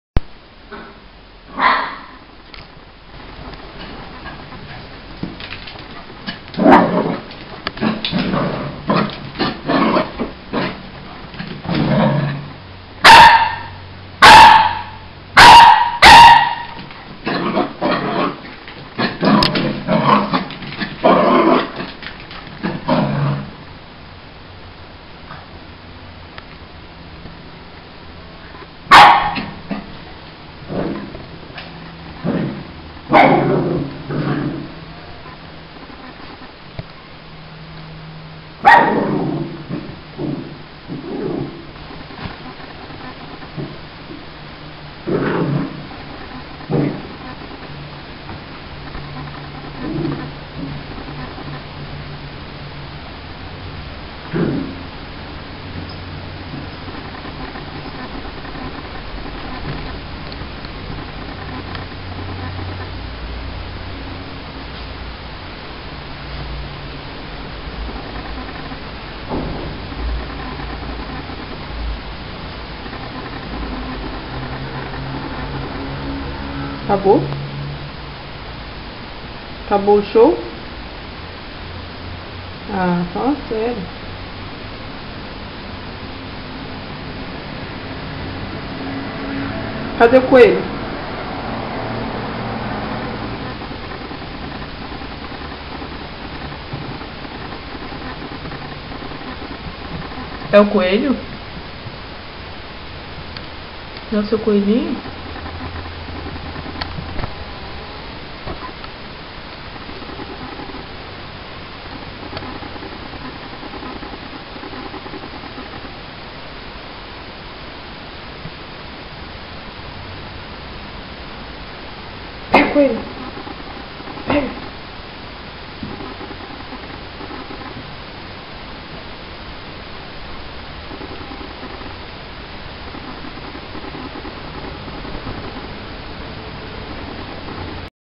minha Lhasa Nala dando a louca rs
lindinha brincando sozinha rs